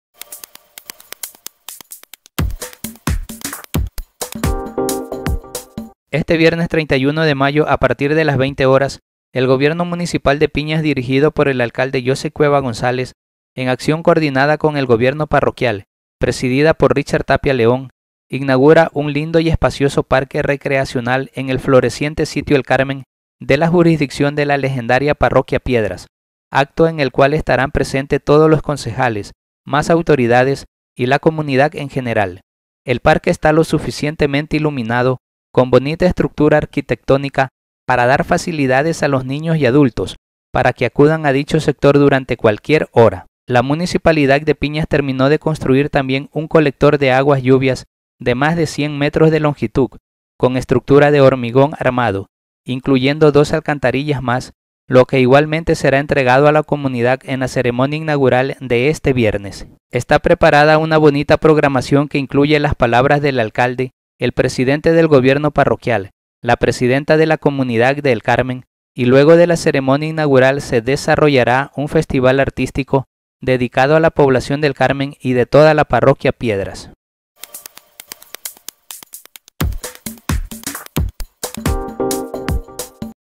Este viernes se inaugura Parque Recreacional y Colector de Aguas Lluvias en El Carmen, Piedras
Este viernes 31 a partir de las 20H00 el Gobierno Municipal de Piñas dirigido por el Alcalde Joseph Cueva González en acción coordinada con el Gobierno Parroquial presidida por Richard Tapia León inaugura un lindo y espacioso parque recreacional en el floreciente sitio El Carmen de la jurisdicción de la legendaria parroquia Piedras, acto en el cual estarán presentes todos los concejales, mas autoridades y la comunidad en general